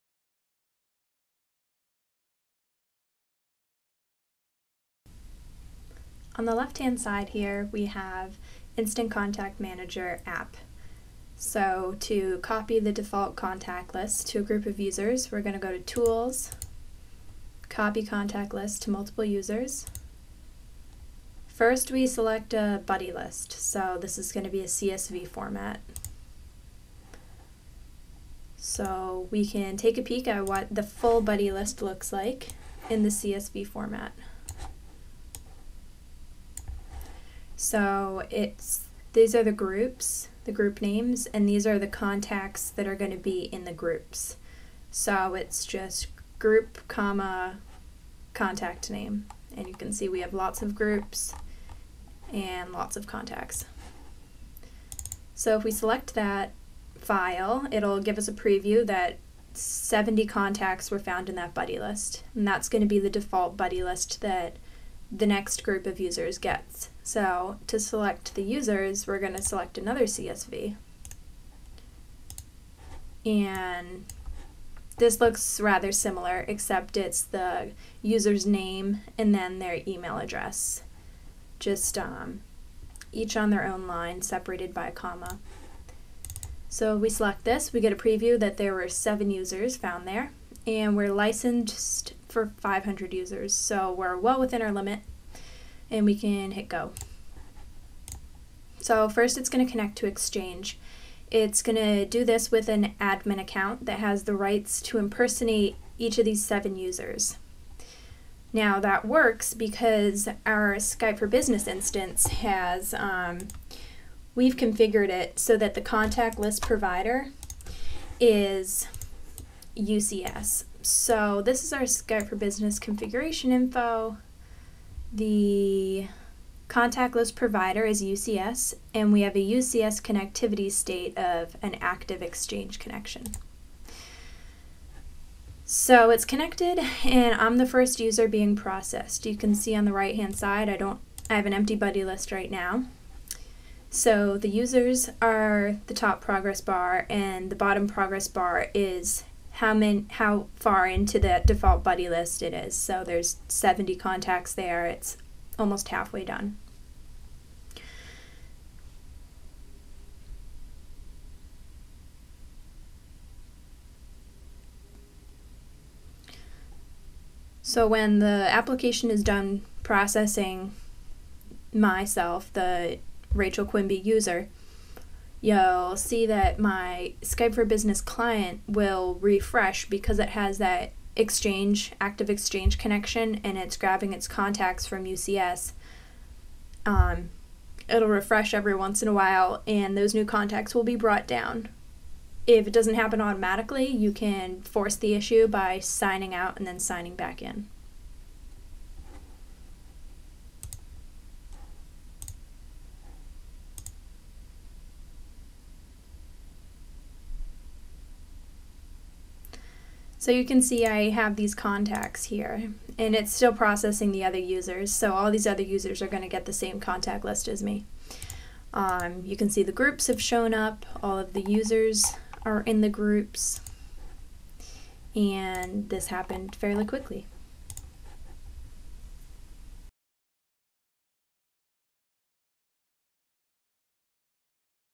How to Push a Default Contact List to a Group - Instant Contact Manager
Instant Contact List Manager provides an enterprise management application to centrally update employee's S4B contact lists as part of a Microsoft Office 365 deployment. However, the application also provides the ability to centrally 'push' contact lists to users without requiring any desktop client installation or configuration.